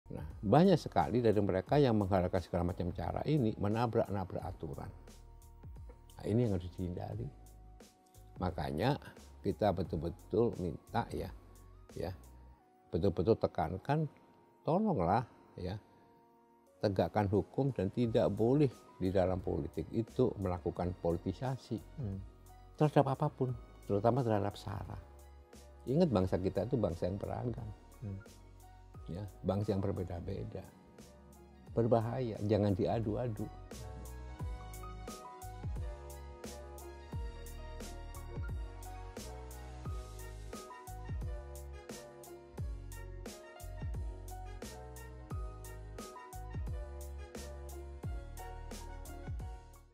Politisi Jangan Adu Rakyat dengan Politisasi Sara dan Agama!
Anggota DPR RI, Djarot Saiful Hidayat memberi pesan kepada para politisi. Jangan mainkan Politisasi Sara dan Agama! Jangan pecah belah masyarakat Indonesia!